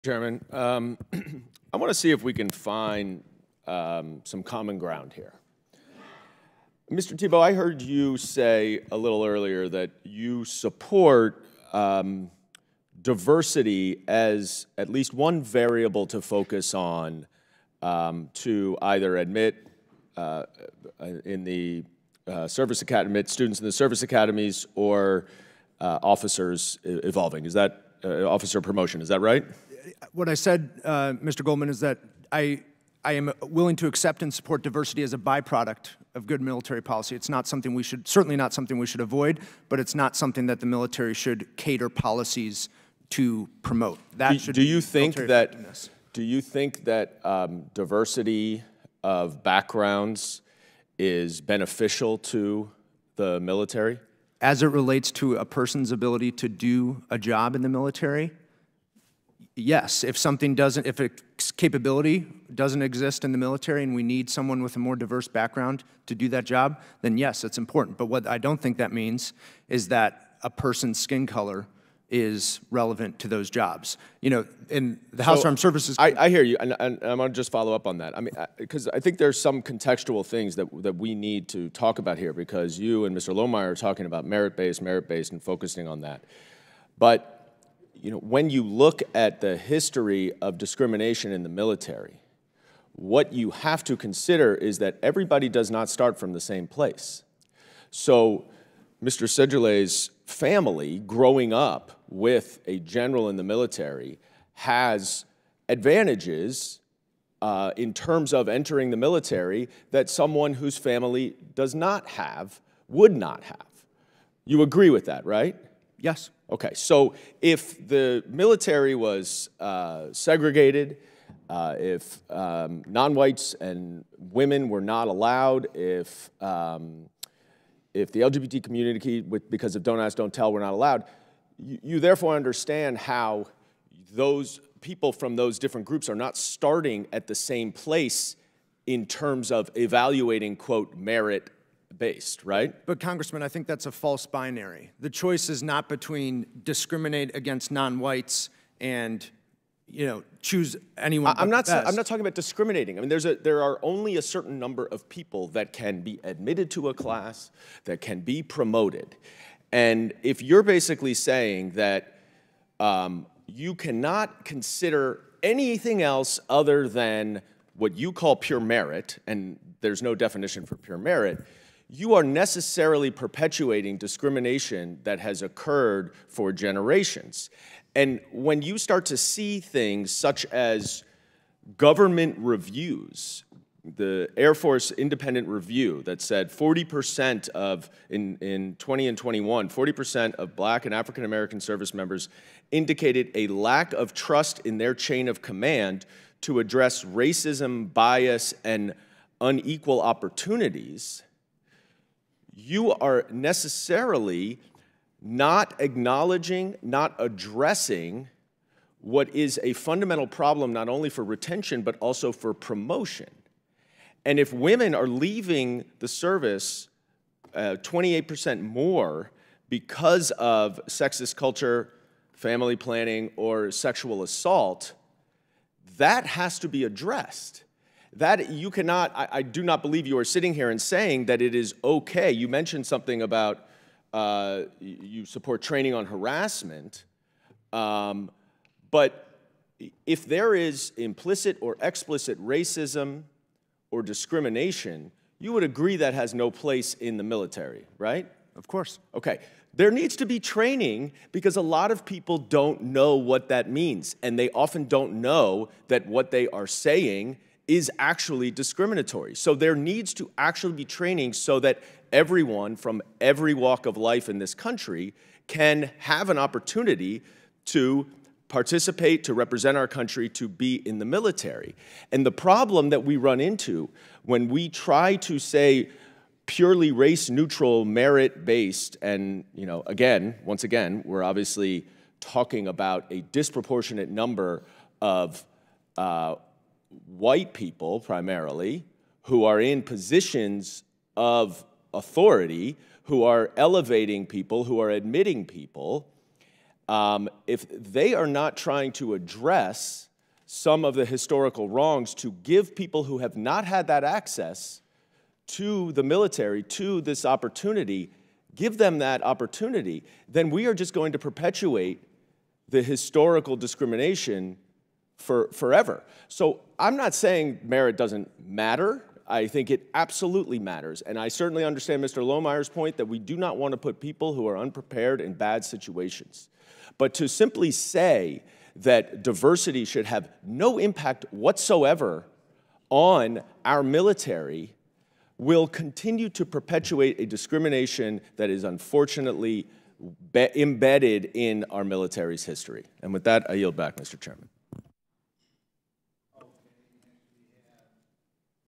'That's A False Binary': Anti-DEI Witness Clashes With Dan Goldman At Hearing On The Military
At a House Oversight Committee hearing last week, Rep. Dan Goldman (D-NY) spoke about Diversity, Equity, And Inclusion in the military.